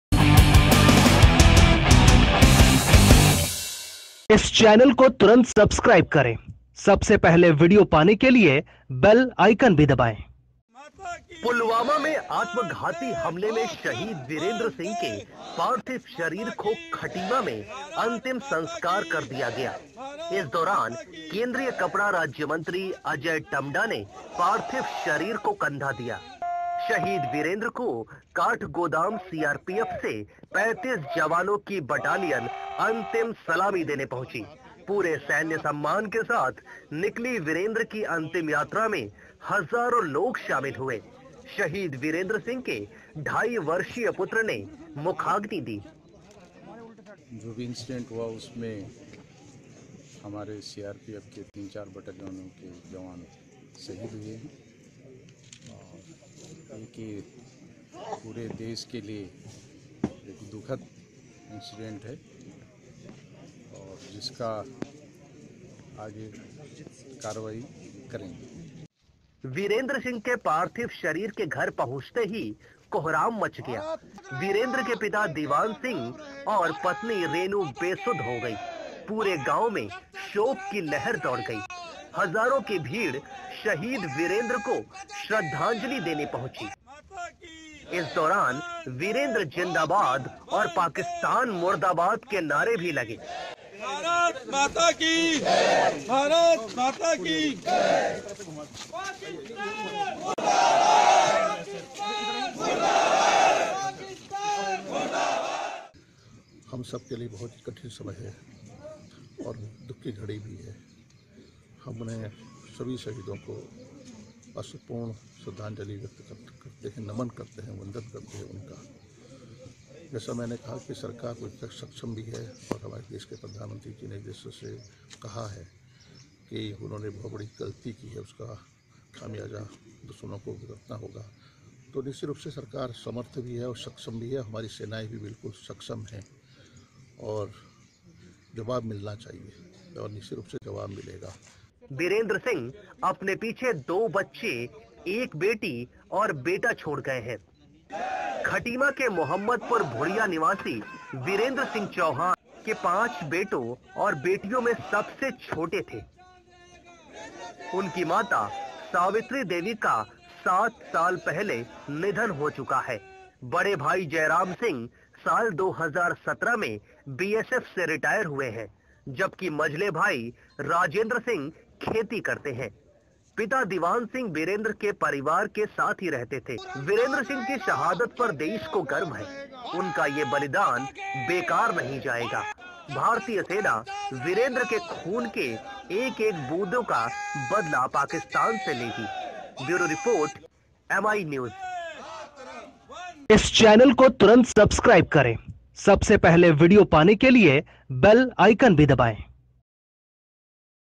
खटीमा के वीरेंद्र सिंह की शहादत को सलाम
पुलवामा में आत्मघाती हमले में शहीद वीरेंद्र सिंह के पार्थिव शरीर को खटीमा में अंतिम संस्कार कर दिया गया...इस दौरान केंद्रीय कपड़ा राज्यमंत्री अजय टम्टा ने पार्थिव शरीर को कंधा दिया..शहीद वीरेंद्र को काठगोदम सीआरपीएफ से 35 जवानों की बटालियन अंतिम सलामी देने पहुंची..पूरे सैन्य सम्मान के साथ निकली वीरेंद्र की अंतिम यात्रा में हजारों लोग शामिल हुए...शहीद वीरेंद्र सिंह के ढाई वर्षीय पुत्र ने मुखाग्निन दी. वीरेंद्र सिंह का पार्थिव शरीर घर पहुंचते ही कोहराम मच गया...वीरेंद्र के पिता दीवान सिंह और पत्नी रेनू बेसुध हो गई. पूरे गांव में शोक की लहर दौड़ गई. हजारों की भीड़ शहीद वीरेंद्र को श्रद्धांजलि देने पहुंची. इस दौरा वीरेंद्र जिंदाबाद और पाकिस्तान मुर्दाबाद के नारे भी लगे. वीरेंद्र सिंह अपने पीछे दो बच्चे एक बेटी और बेटा छोड़ गए हैं. खटीमा के मोहम्मदपुर भुड़िया निवासी वीरेंद्र सिंह दीवान सिंह के पांच बेटों और बेटियों में सबसे छोटे थे..उनकी माता सावित्री देवी का सात वर्ष पूर्व निधन हो चुका है। बड़े भाई जयराम सिंह वर्ष 2017 में बीएसएफ से रिटायर्ड हुए हैं..जबकि मझले भाई राजेंद्र सिंह खेती करते हैं...पिता दीवान सिंह वीरेंद्र के परिवार के साथ ही रहते थे..